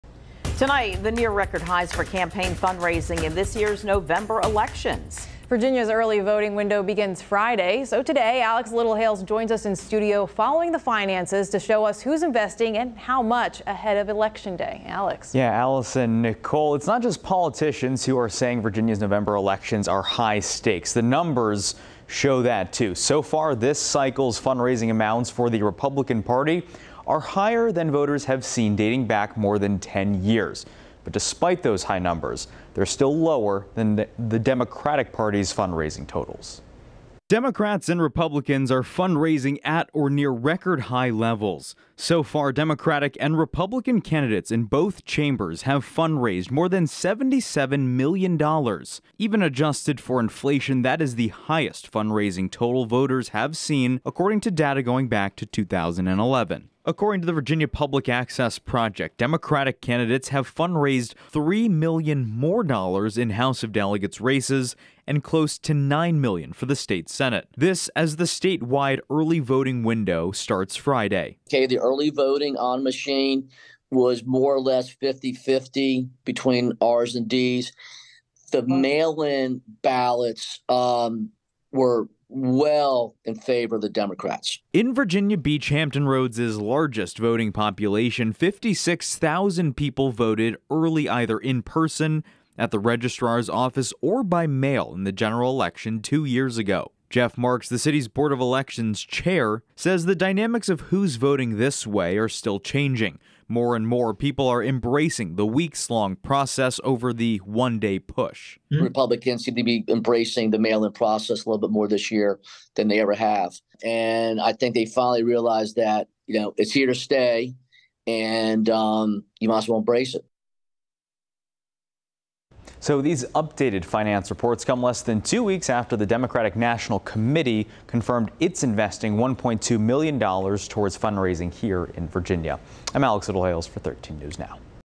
Fundraising in Virginia's 2023 November Elections sets record high for both chambers
This cycle's fundraising amounts for the Republican party are higher than voters have seen before. However, they're still lower than the Democratic party's.